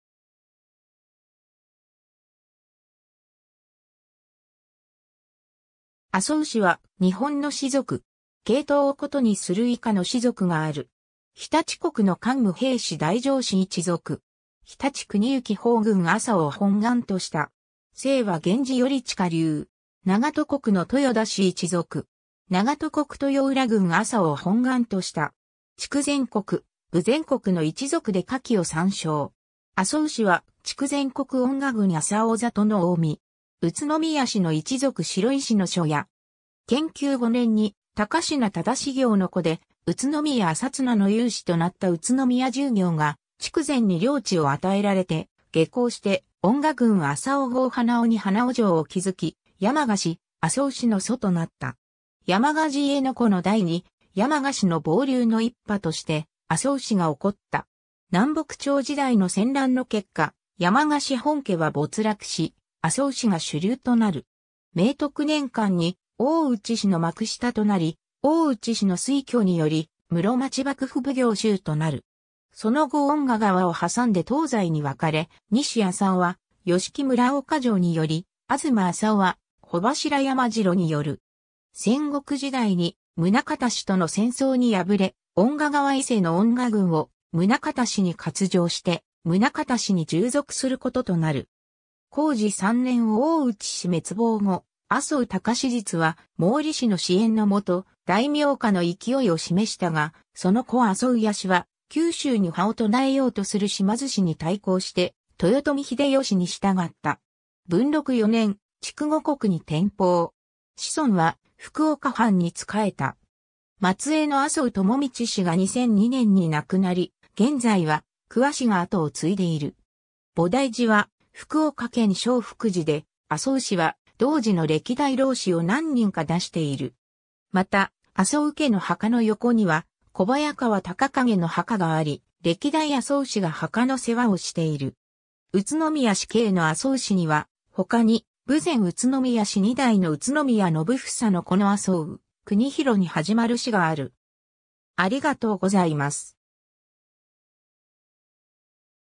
麻生氏
麻生氏（あそうし）は、日本の氏族。
系統を異にする以下の氏族がある。
 常陸国の桓武平氏大掾氏一族。
常陸国行方郡麻生を本貫とした（常陸麻生氏）。
 清和源氏頼親流（大和源氏）。
 長門国の豊田氏一族。
長門国豊浦郡麻生を本貫とした（長門麻生氏）。
 筑前国、豊前国の一族で下記を参照。
 麻生（あそう）氏は、筑前国遠賀郡麻生郷の大身。
宇都宮氏の一族城井氏（豊前宇都宮氏）の庶家。
 建久5年に、高階忠業の子で宇都宮朝綱の猶子となった宇都宮重業が筑前に領地（約1,000町）を与えられて、下向して遠賀郡麻生郷花尾に花尾城（はなおじょう）を築き、山鹿氏・麻生氏の祖となった。
山鹿時家の子の代に、山鹿氏の傍流の一派として麻生氏が興った。
南北朝時代の戦乱の結果、山鹿氏本家は没落し、麻生氏が主流となる。
 明徳年間に大内氏の幕下となり、大内氏の推挙により室町幕府奉行衆となる。
その後遠賀川を挟んで東西に分かれ、西麻生は吉木村岡城に拠り、東麻生は帆柱山城に拠る。
 戦国時代に、宗像氏との戦争に敗れ、遠賀川以西の遠賀郡を宗像氏に割譲して、宗像氏に従属することとなる。
 弘治3年（1557年）大内氏滅亡後、麻生隆実は毛利氏の支援のもと、大名化の勢いを示したが、その子麻生家氏は九州に覇を唱えようとする島津氏に対抗して、豊臣秀吉に従った。
 文禄4年（1595年）、筑後国に転封。
子孫は福岡藩に仕えた。
 末裔の麻生朝道氏が2002年に亡くなり、現在は久和氏が跡を継いでいる。
 菩提寺は福岡県聖福寺で、麻生氏は同寺の歴代老師を何人か出している。
 また、麻生家の墓の横には小早川隆景の墓があり、歴代麻生氏が墓の世話をしている。
 宇都宮氏系の麻生氏には、他に豊前宇都宮氏2代の宇都宮信房の子の麻生国弘にはじまる氏がある。
 （筑前国嘉麻郡立岩村 庄屋の一族、第92代内閣総理大臣麻生太郎の実家）